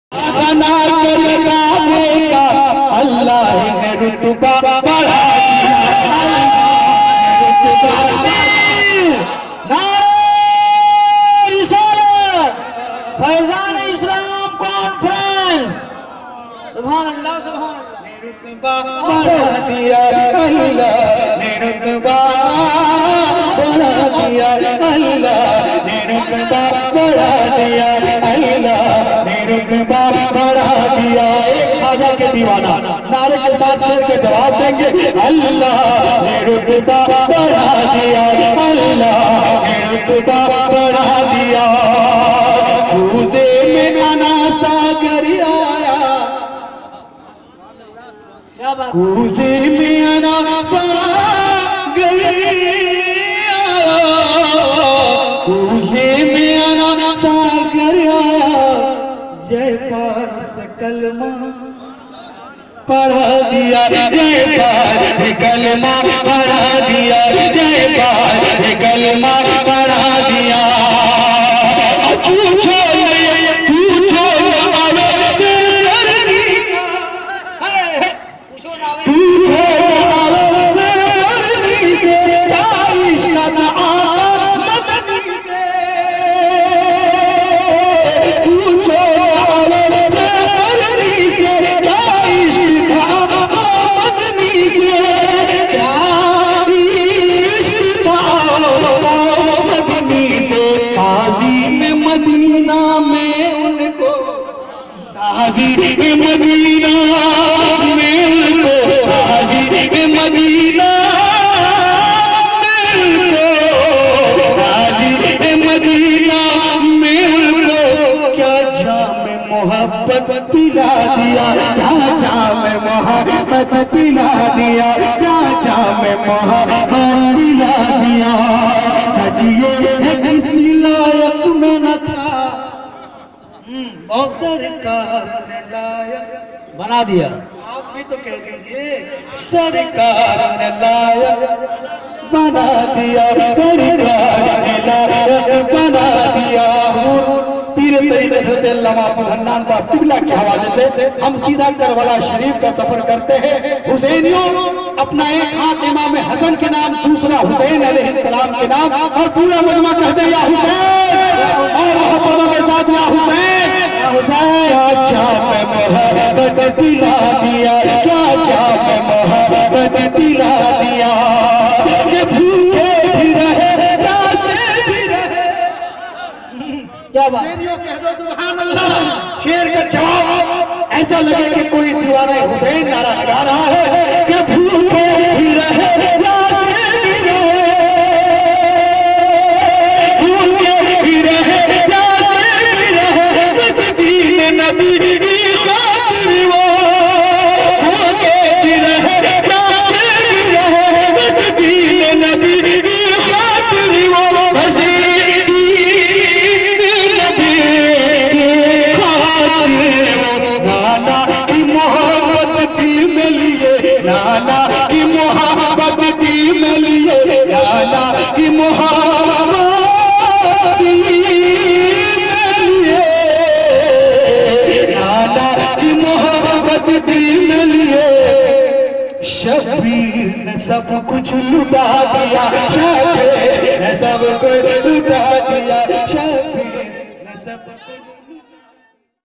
mojahid Raza Mazhar Ne Hangama Kar diya
jamshed pur Urse Qaide Millat Allama Arshadul Quadari Ke Moqe Pe Shandar Naat Goyi